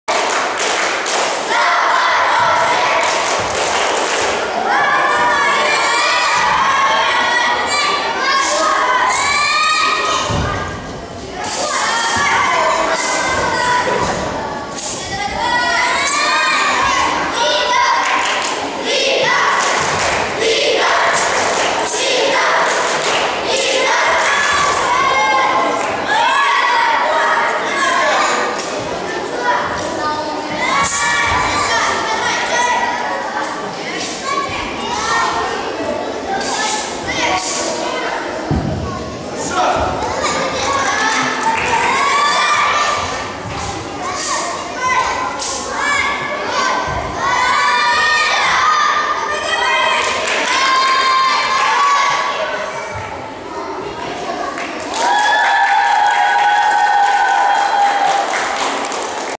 Кубок Азовского моря 2014. Соболь Лидия. Цзянь шу (чуцзи)
Кубок Азовского моря 2014, 13-15 июня, г. Бердянск